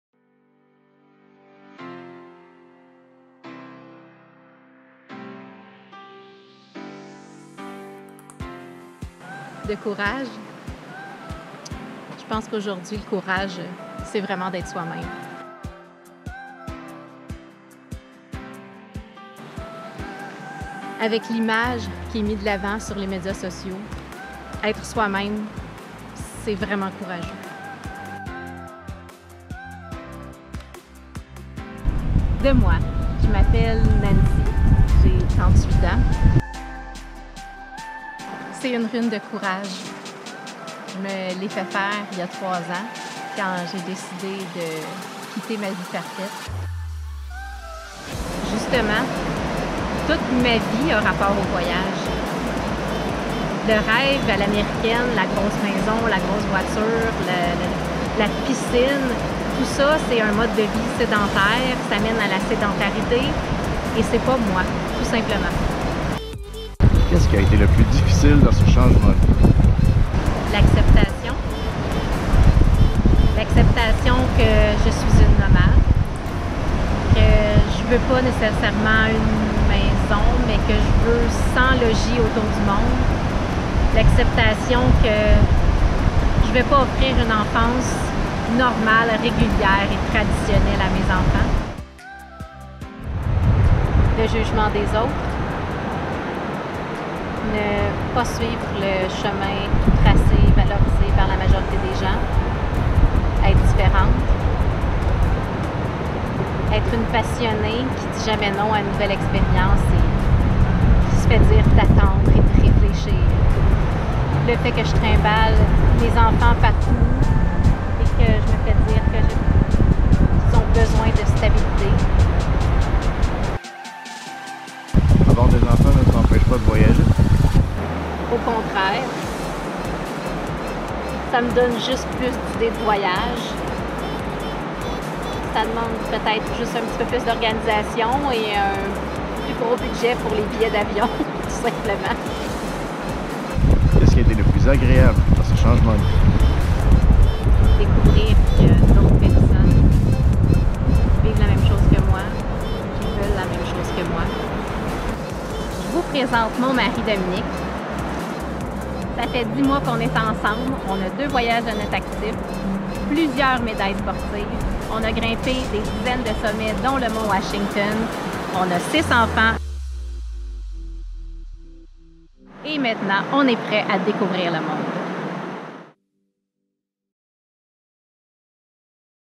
Le Courage du Nomade / Nomad's Courage HD
Entrevue de Nancy Gadoury sur le courage de nos jours.
Interview with Nancy Gadoury on courage.